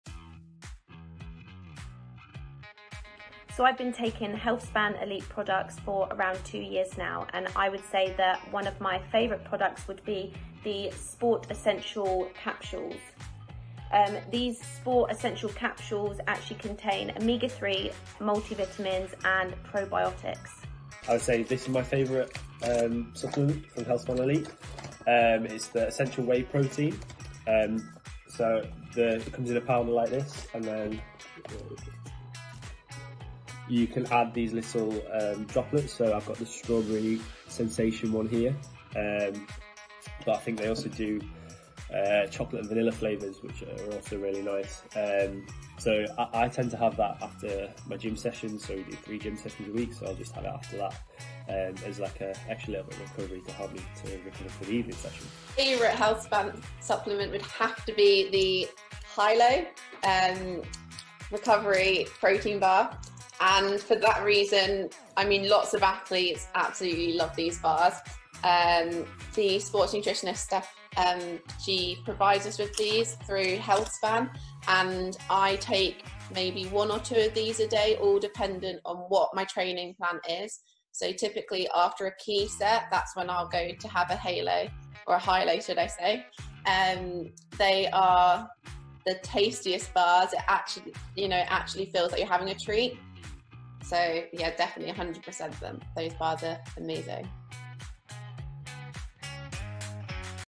Robyn Birch, Luke Greenbank and Hannah Russell share their Favourite Healthspan Elite Supplement
British Swiming's Robyn Birch, Luke Greenbank and Hannah Russell share their favourite supplements from Official Vitamin and Supplement Partner Healthspan Elite.